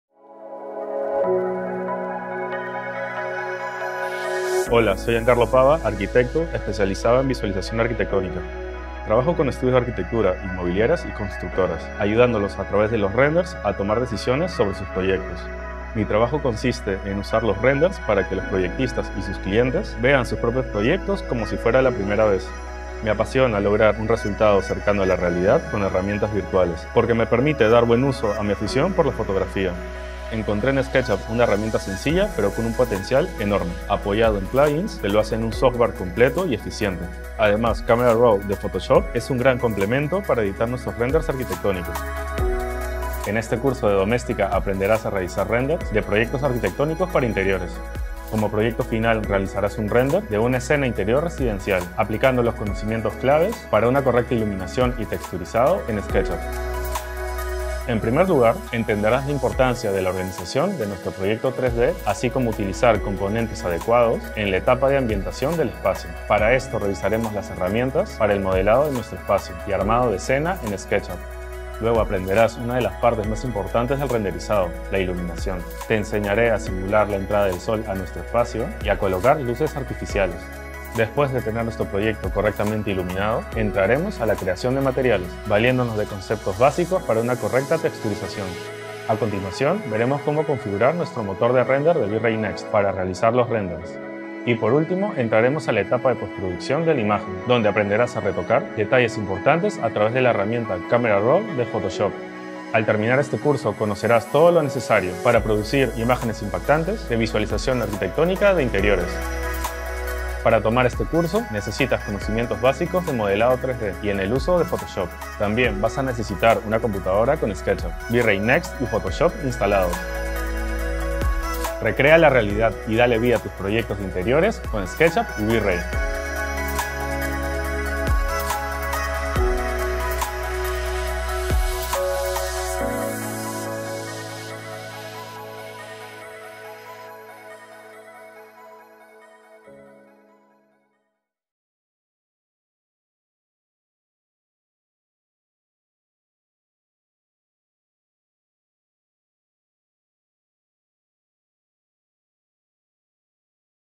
CURSO ONLINE Render de interiores con SketchUp y V-Ray Next de Giancarlo Pava
Aprende a trabajar correctamente la iluminación y los materiales para crear renders impactantes de proyectos arquitectónicos

Trabajar profesionalmente cada detalle de un render arquitectónico te permite lograr resultados únicos y emocionantes que reflejan la realidad. Giancarlo Pava, arquitecto especializado en ArchViz, te enseñará todo lo necesario para darle vida a tus proyectos de interiores con SketchUp y V-Ray.

En este curso aprenderás a producir renders de proyectos arquitectónicos de interiores. Descubrirás los conocimientos clave de la visualización arquitectónica y trabajarás luces, texturas y materiales para crear una imagen final impactante.